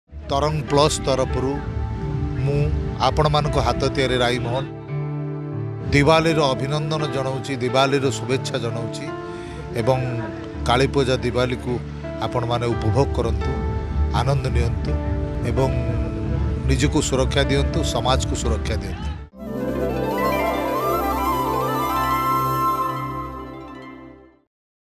Diwali Special 2021 | Raimohan | Happy Diwali | Tarang Plus
The one and only Raimohan along with Tarang Plus wish you a very happy and safe Diwali. May your life be filled with joy, health and prosperity on this auspicious day.